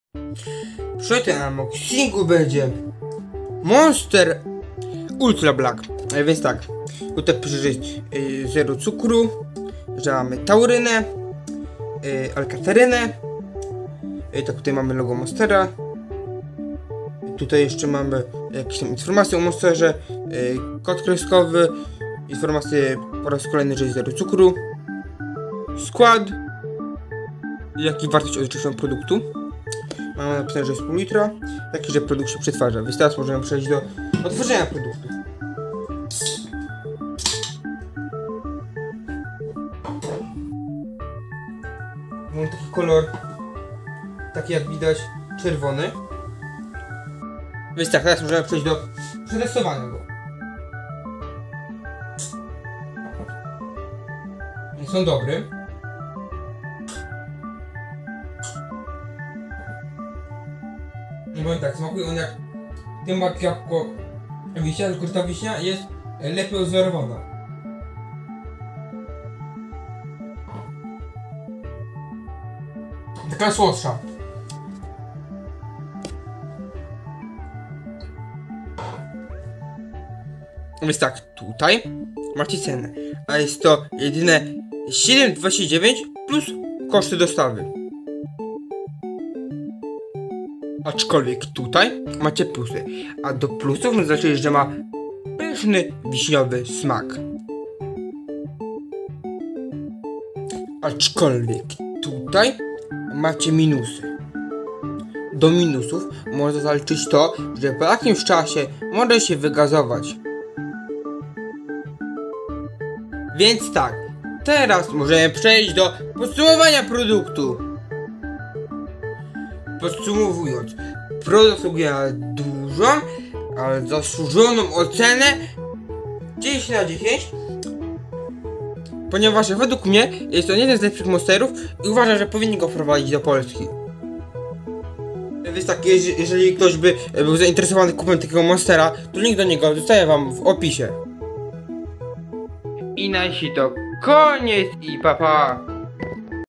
Test Monstera (ULTRA BLACK)
Piję i oceniam w skali od 1 do 10